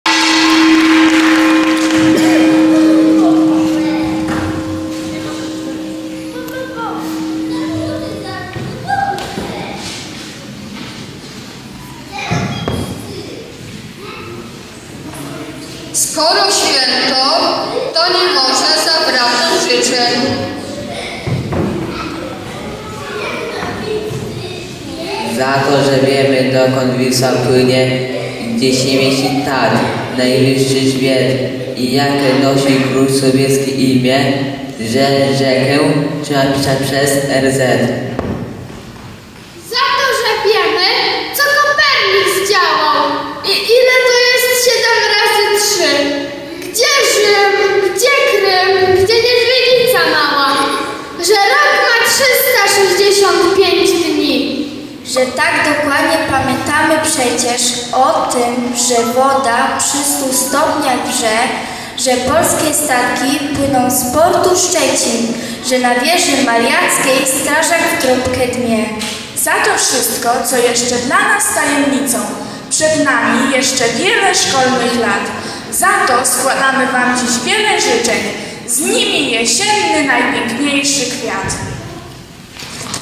Skecz 4
Występ uczniów Zespołu Szkół w Koronowie z okazji 250 rocznicy powołania Komisji Edukacji Narodowej.